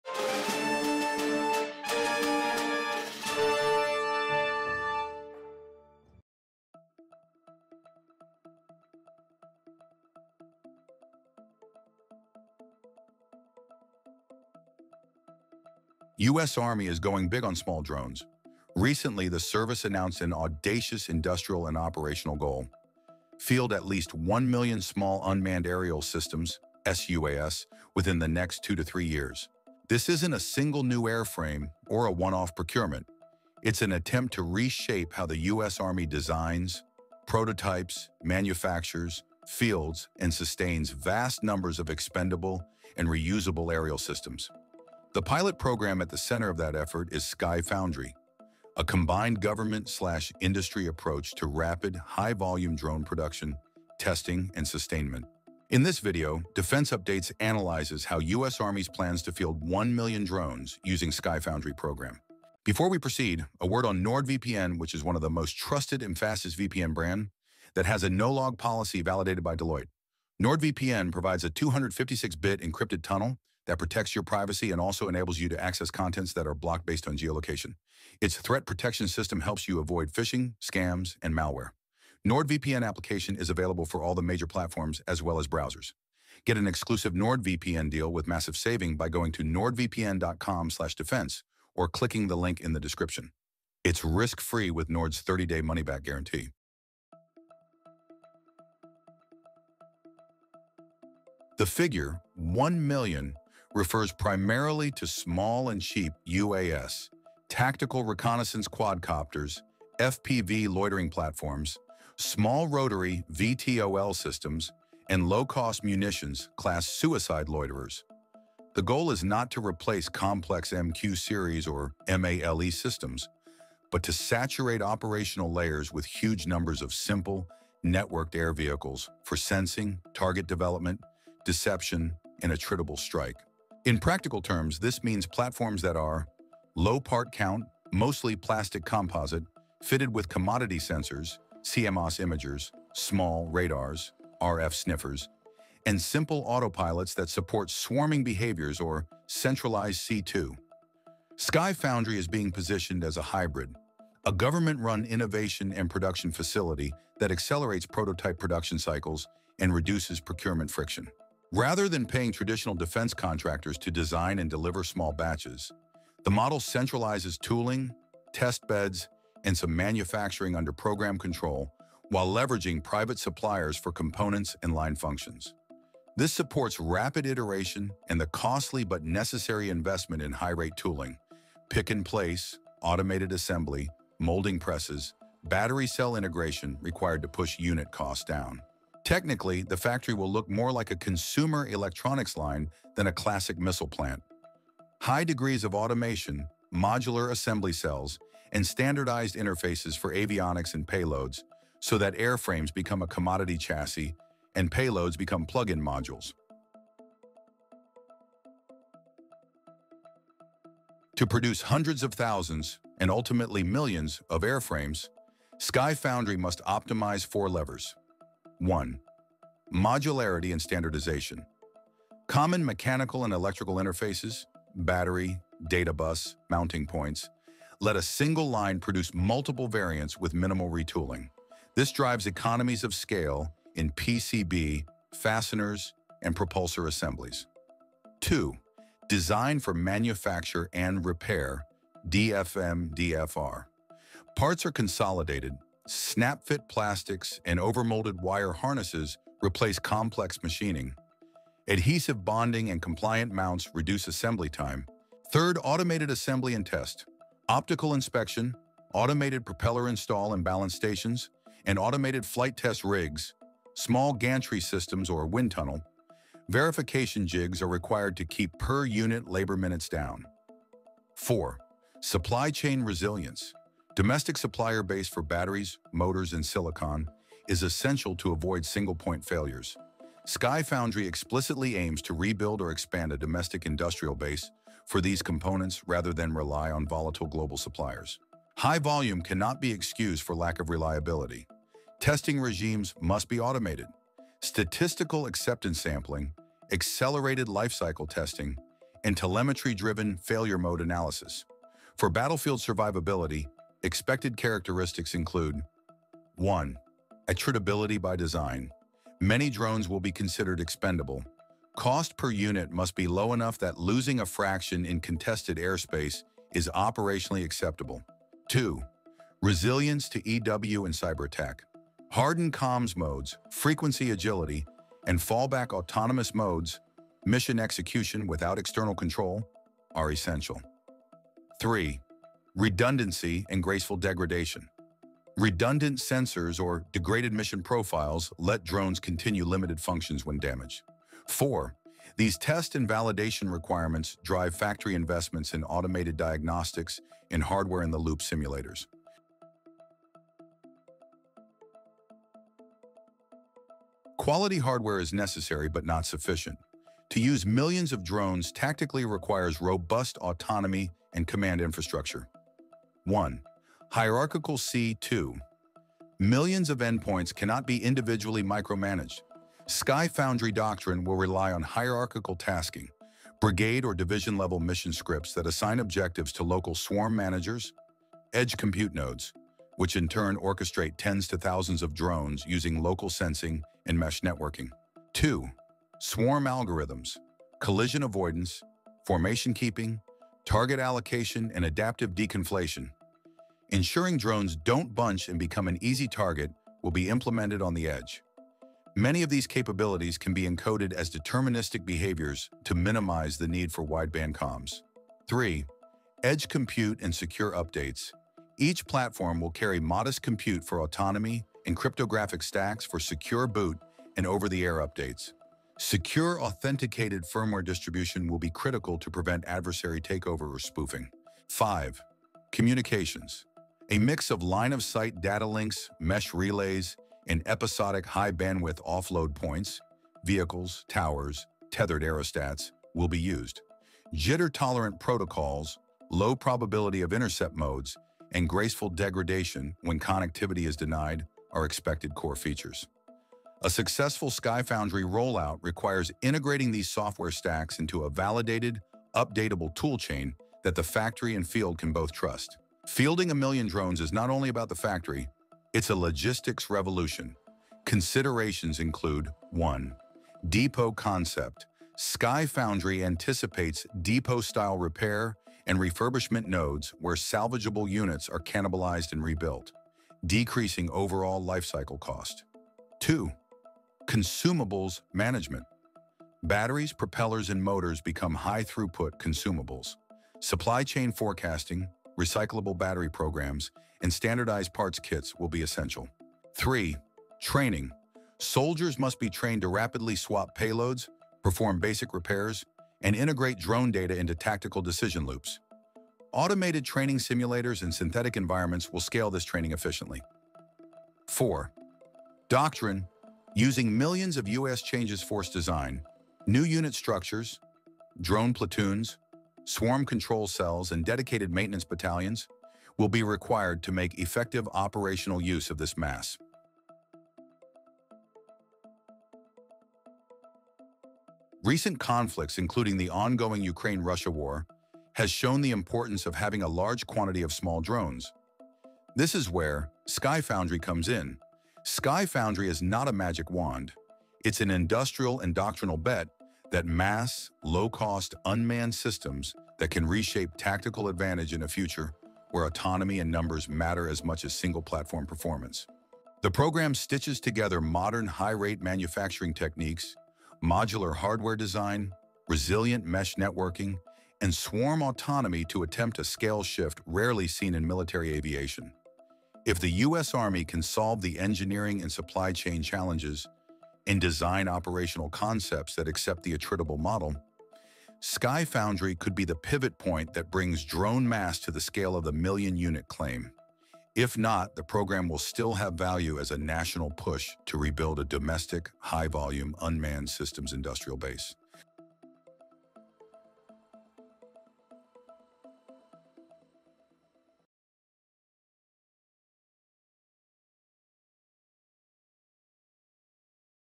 How US Army’s plans to field 1 million drones using SkyFoundry program ?
US Army is going big on small drones.
Recently, the service announced an audacious industrial and operational goal: field at least one million small unmanned aerial systems (sUAS) within the next two to three years.
This isn’t a single new airframe or a one-off procurement: it’s an attempt to reshape how the US Army designs, prototypes, manufactures, fields, and sustains vast numbers of expendable and reusable aerial systems. The pilot program at the center of that effort is SkyFoundry — a combined government/industry approach to rapid, high-volume drone production, testing and sustainment.

In this video, Defense Updates analyzes how US Army’s plans to field 1 million drones using SkyFoundry program ?

Chapters:
0:00 TITLE
00:11 INTRODUCTION
01:02 SPONSORSHIP - NordVPN
03:26 CONCEPT
05:48 THE PROBLEM
08:36 ANALYSIS